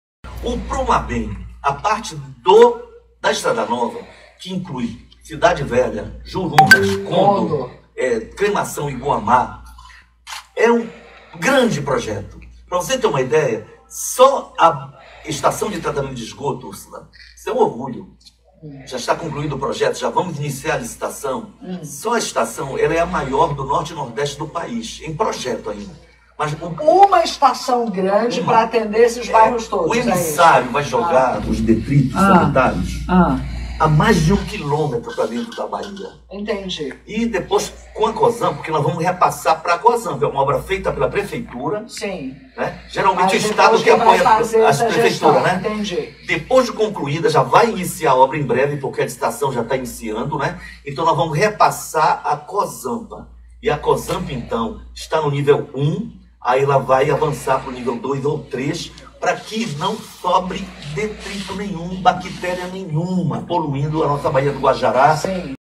Saneamento - Prefeito de Belém anuncia a maior Estação de Esgoto do Norte e Nordeste do Brasil!
Em entrevista à Radio Clube di Pará, apresentado pela Secretaria de Cultura do Pará, Ursula Vidal, o Prefeito de Belém Edmilson Rodrigues anuncia a maior Estação de Tratamento de Esgoto do Norte e Nordeste do Brasil!